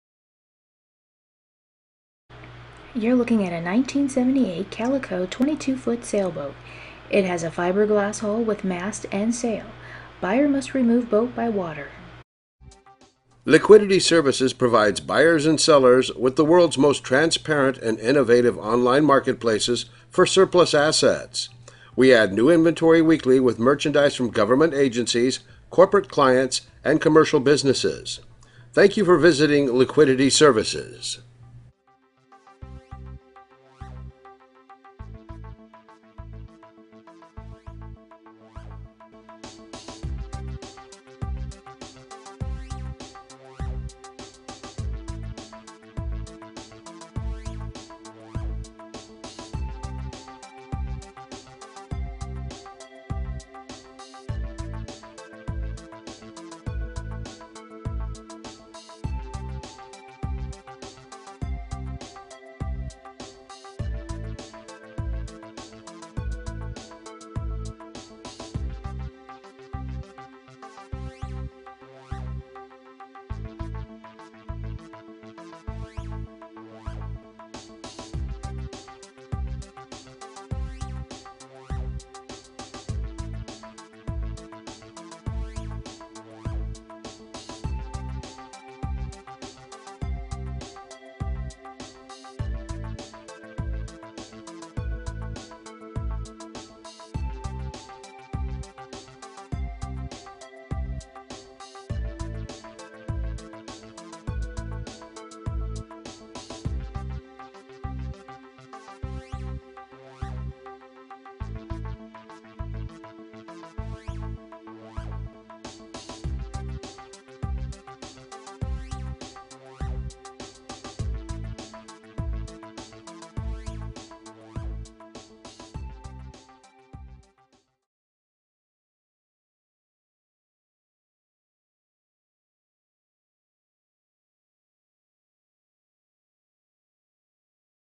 1978 22 ft. Catalina C-22k Sailboat on GovLiquidation.com
- This 1978 22 ft. Catalina C-22k Sailboat has been sold.  The sailboat was located in Coronado, CA.

Hull #8237 with main sail and jib.